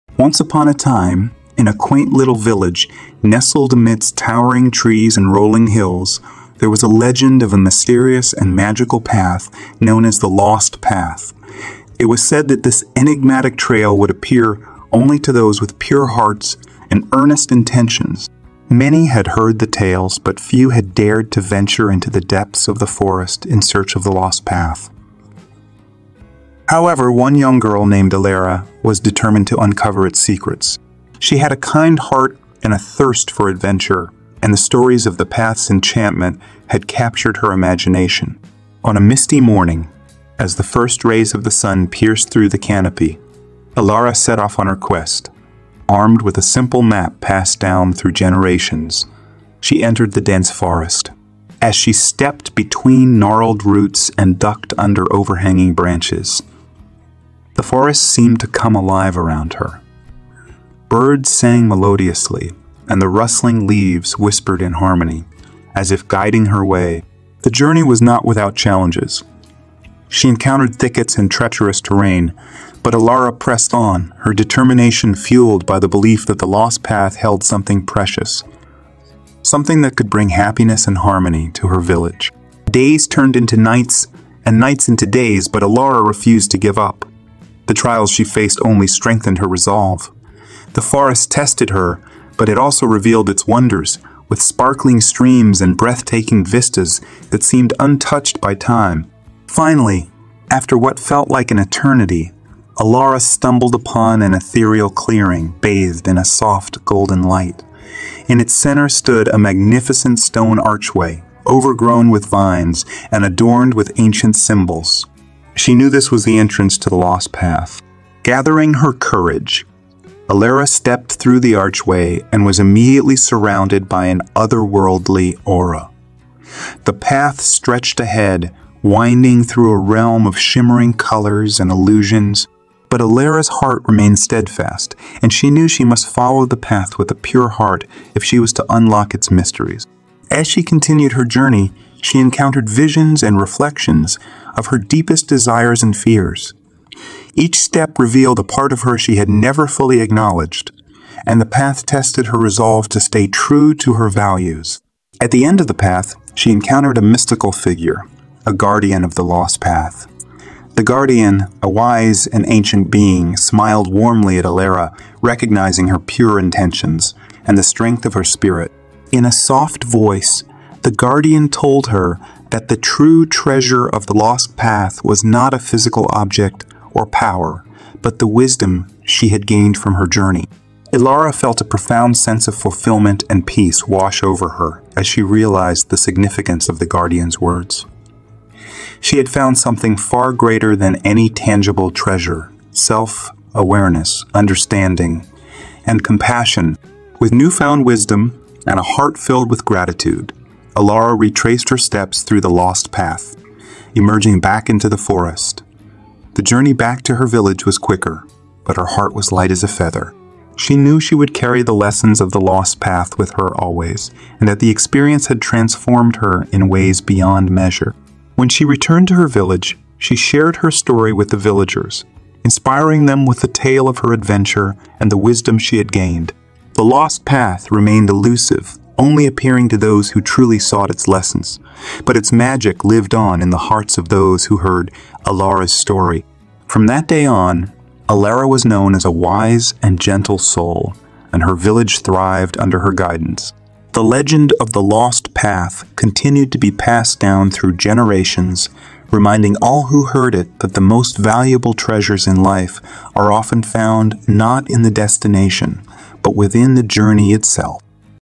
The Lost Path: An Interesting Short Story
This is a captivating adventure of magic and mystery  "The Lost Path." In this heartwarming short story, we follow the courageous young girl, Elara, as she embarks on a quest that will forever change her life and her village.